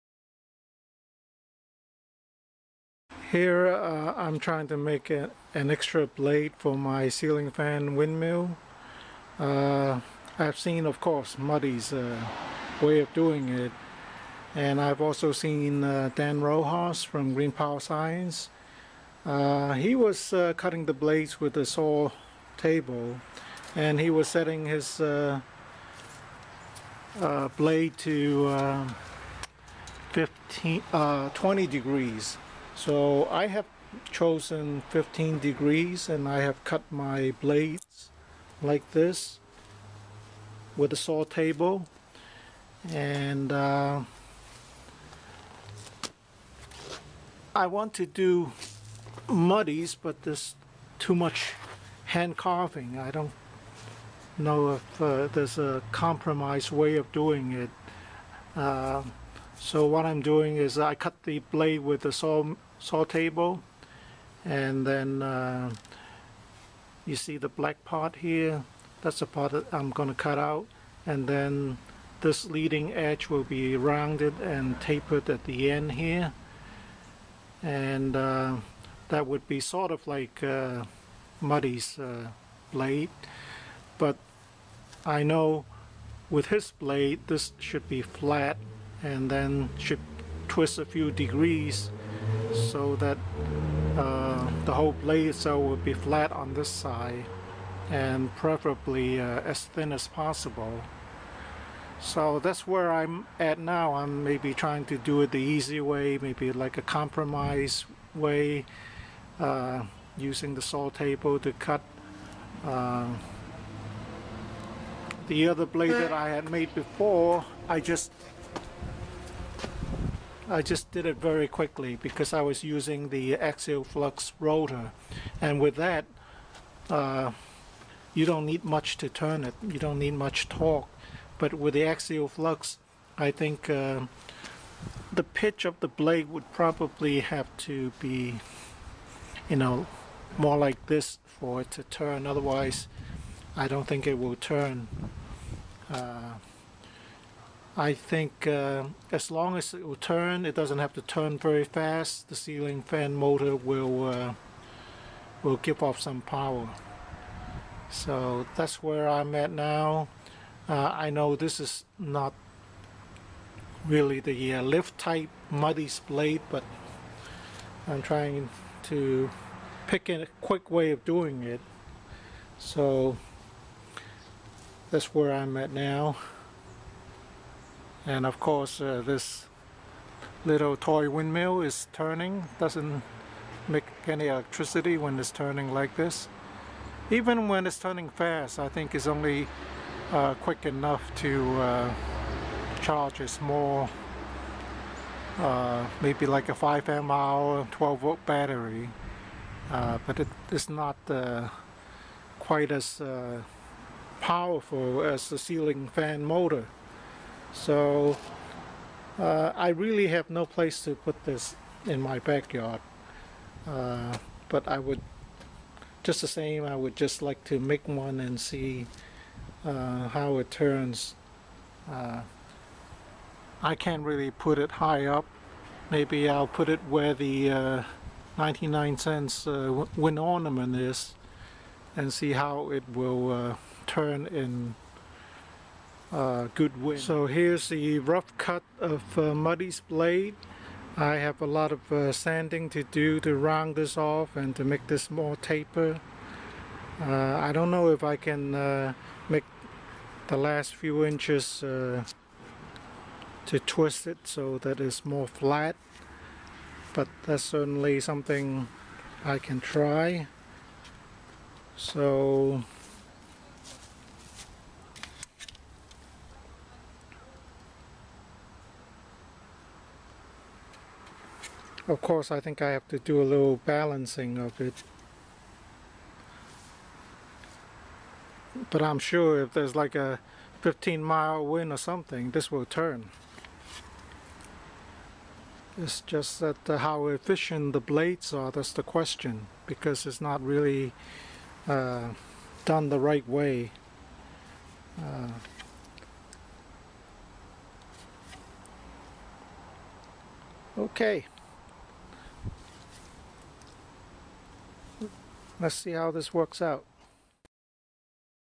Making blades for the ceiling fan motor wind turbine .mp4
To save time in making the blade for my ceiling fan motor , I used my saw table to make this 4' blade using Dan Rojas' video as a guide. He cut the blade at 20 degrees. I chose a 15 degree cut and I tried to make the blade into a more aerodynamic (airfoil)  shape as shown by Muddy.  I did not make the blade twist at the tip so it won't be as efficient as Muddy's blade.  The table saw is a dangerous tool so be extra careful not to hurt yourself.